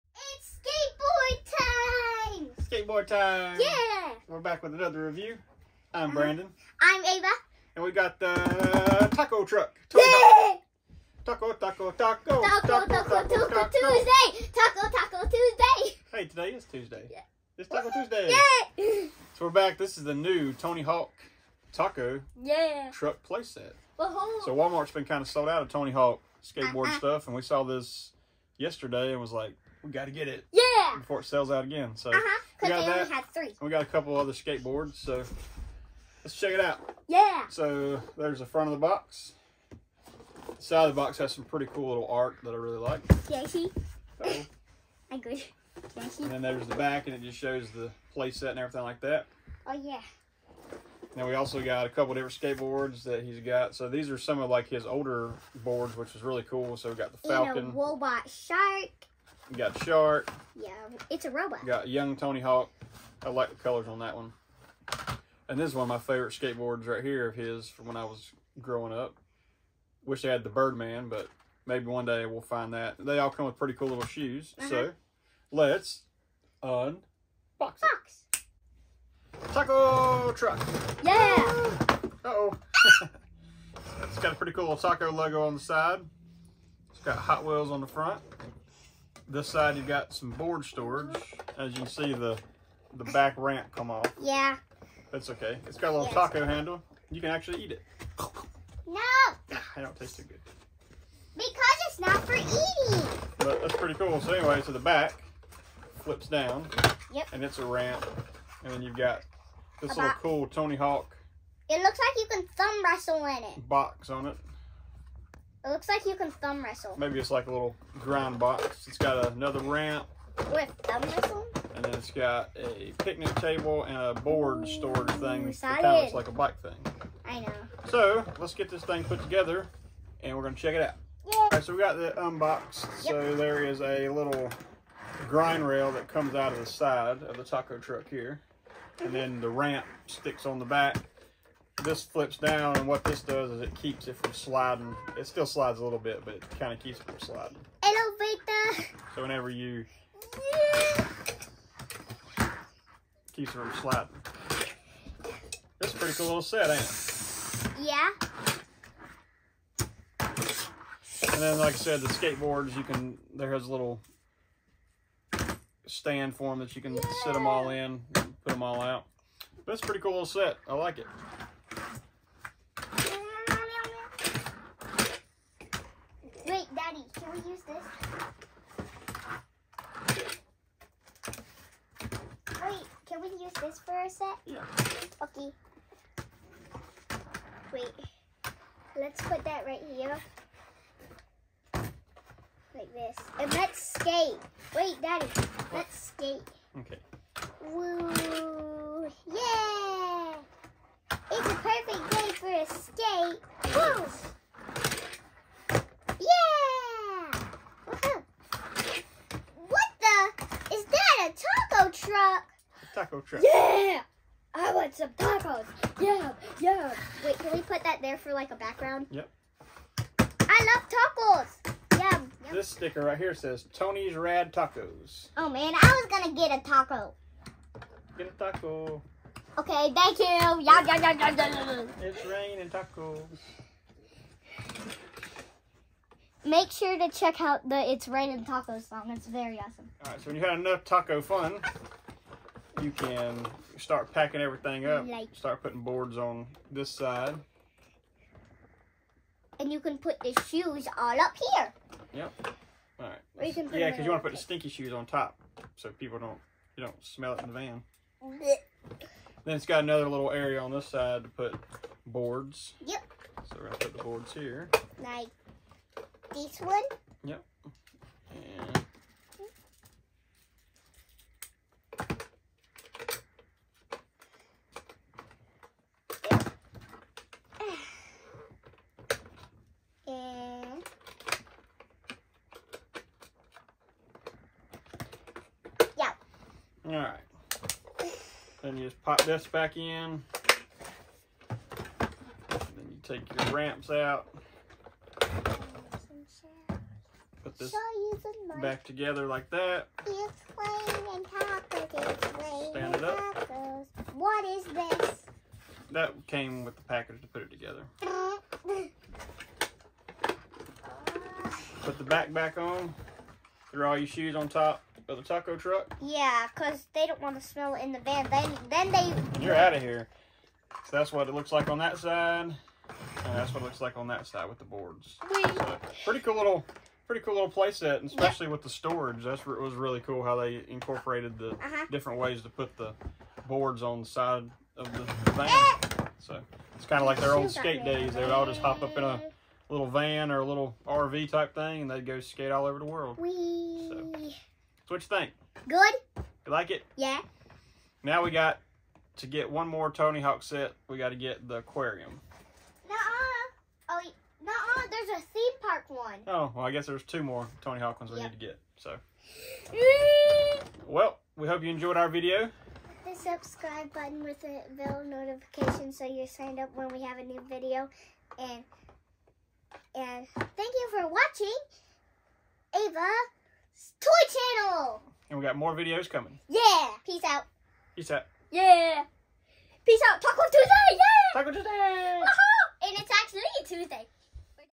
Hot wheels Skate Tony Hawk Taco Truck play set unboxing
Checking out the new Hot Wheels Tony Hawk Taco truck play set